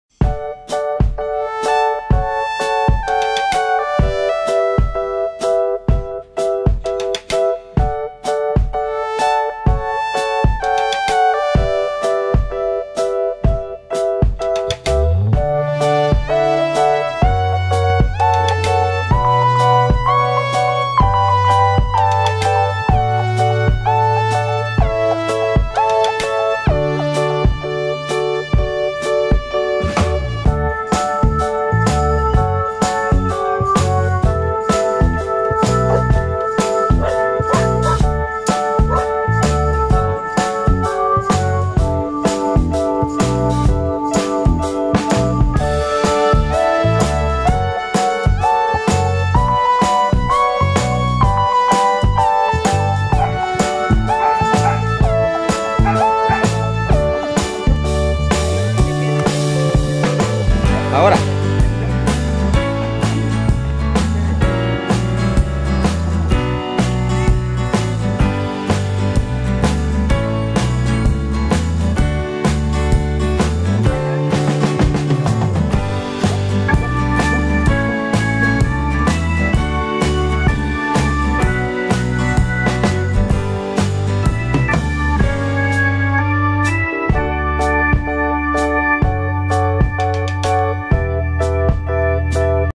divirtiendome con mis macotas en el cafetalon
Pues entrenando a mi pero en santa tecla pues queriendo hacer que sea educado y pacifico con mucho ejercicio  y luego empezamos el entrenamiento
pues tratro de divertirme con mis mascota lo mejor que puedo y darles la mejor alimentación que puedo y sus vitamina